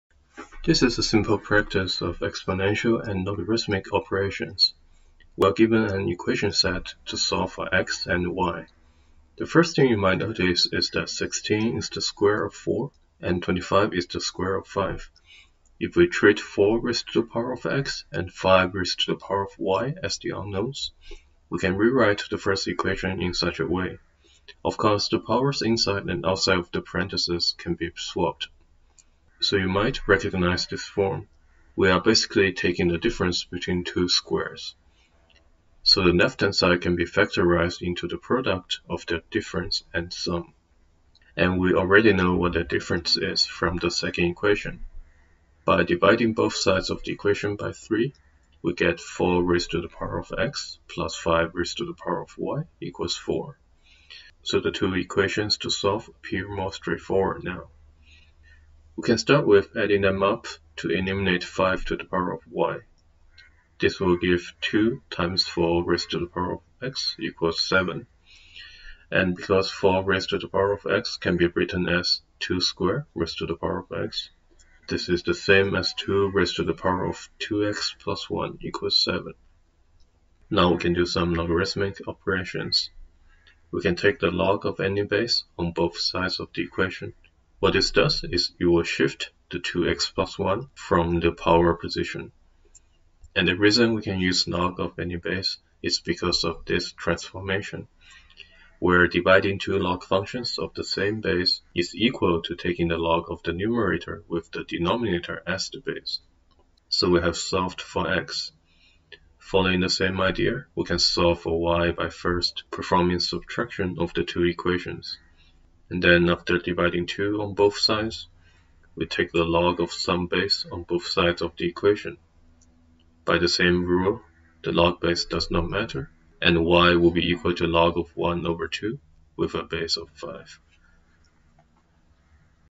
A-Math: Some basis operations of exponential/logarithmic functions #2
An introductory problem to solve for two unknowns with exponential and logarithmic operations.